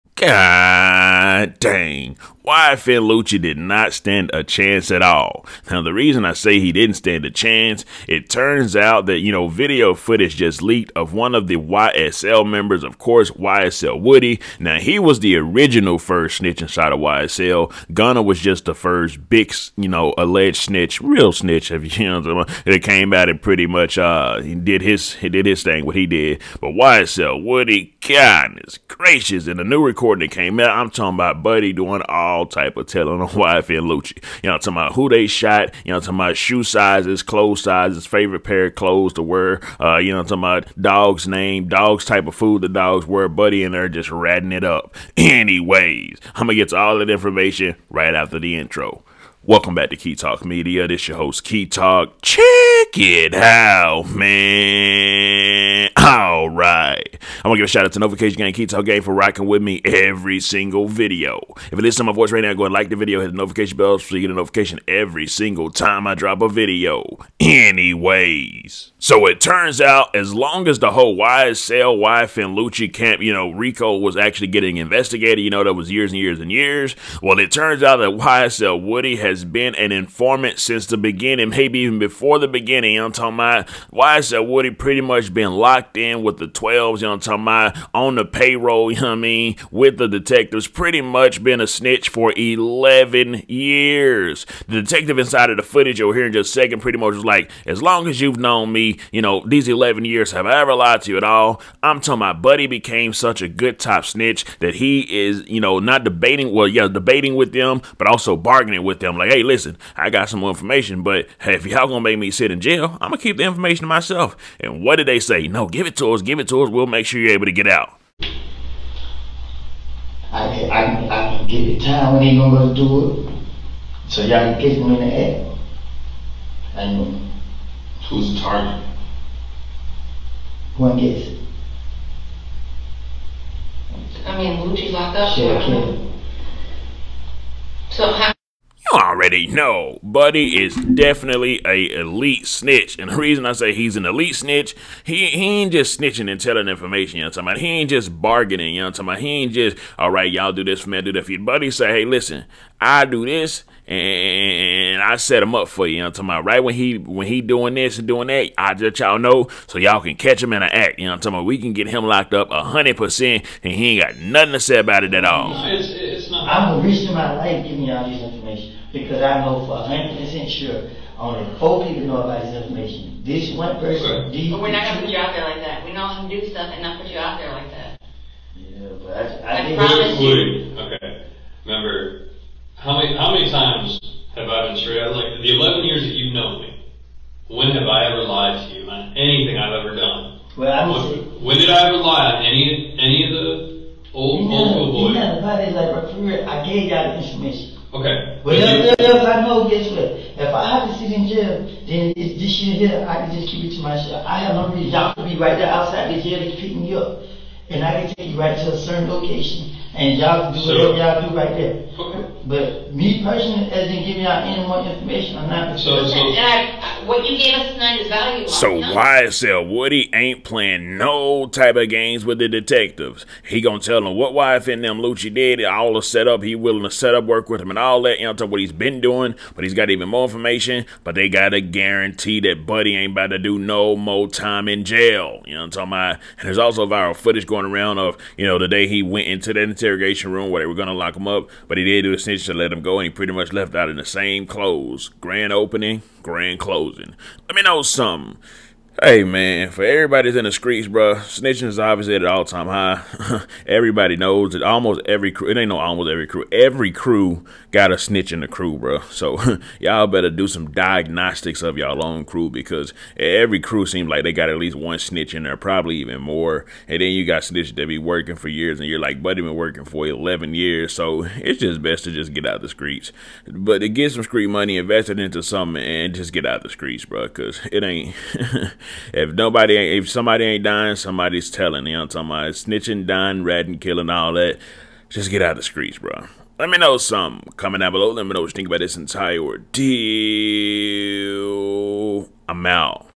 YFN Lucci DiDNT STAND a CHANCE! Young Thug YSL GANG had a PRO RAT 11Yrs, YSL Woody INTERROGATION VID
This is for and Entertainment & satire